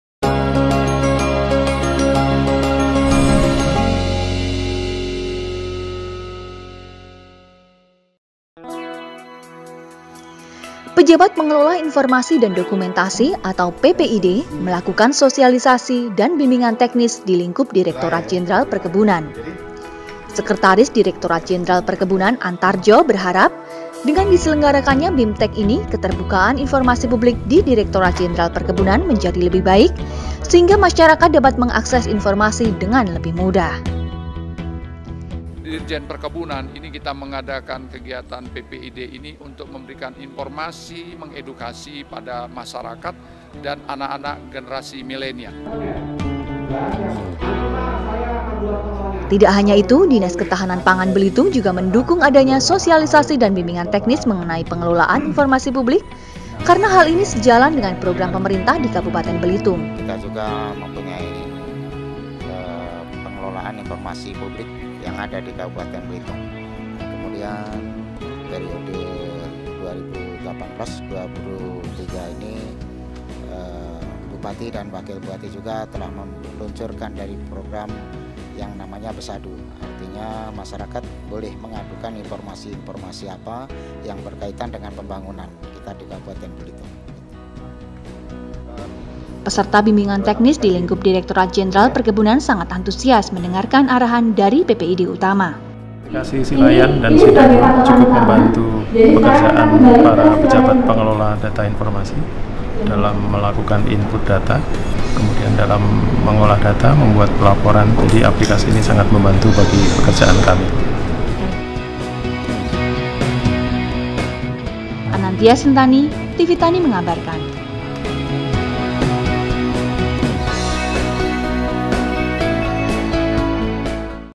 DIREKTORAT JENDERAL PERKEBUNAN SIAP UNTUK MENGEDEPANKAN INFORMASI PUBLIK
Bimbingan Teknis PPID menjadi kiat Direktorat Jenderal Perkebunan dalam memajukan  keterbukaan informasi publik dan kecepatan dalam sharing informasi untuk masyarakat luas..